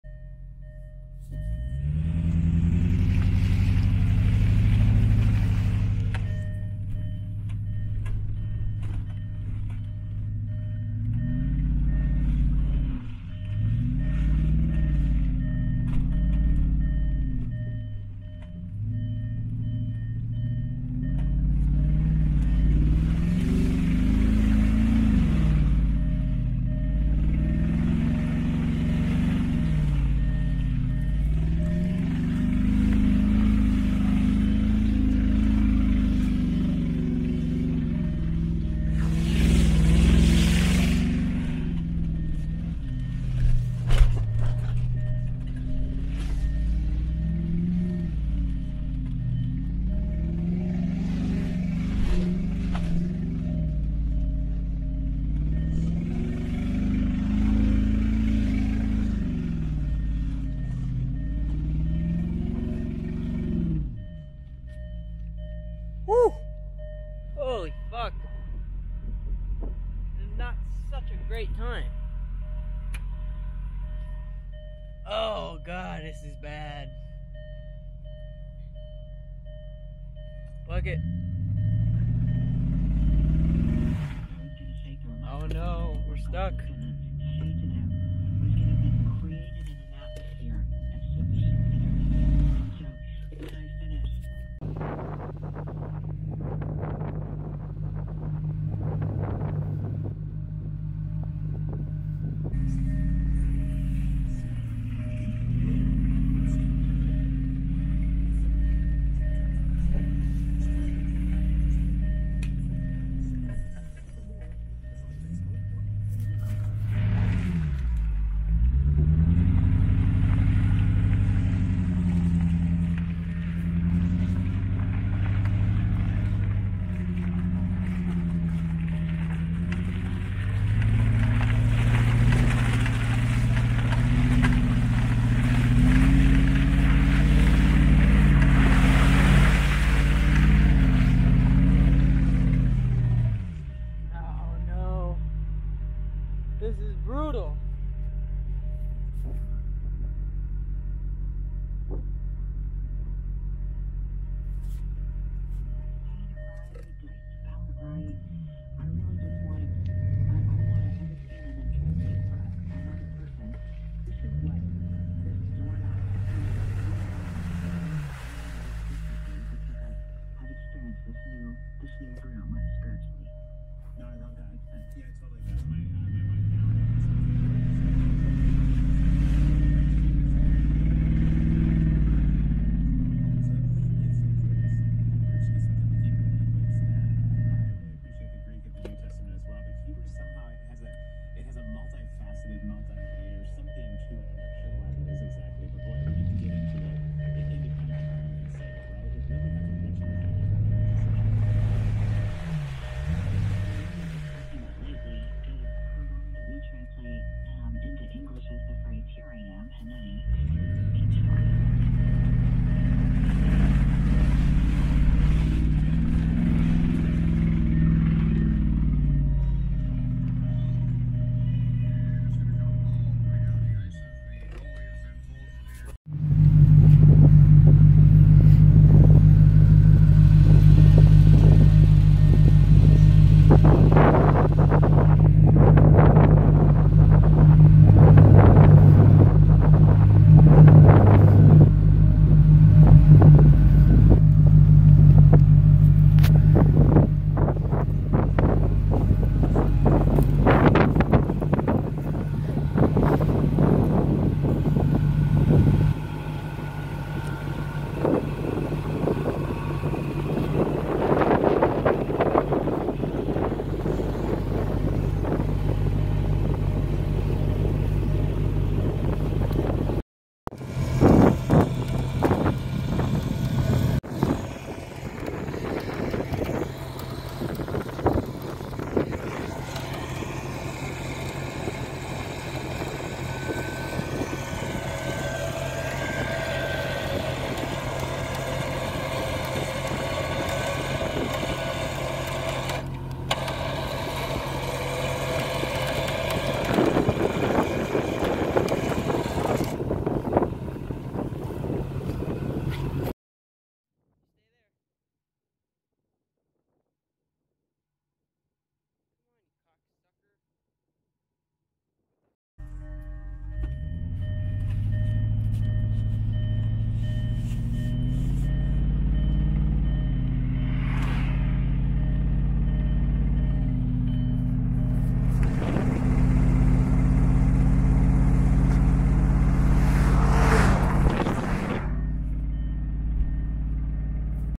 Boolin threw the sauce with my woof - Muddy Spring MN
Northern Minnesota Iron Range 1999 land Rover Discover II Mud Bog. Spring logging trail with my boy. Overland 2021.

Shot using a DJI Osmo Pocket
Edited on Davicini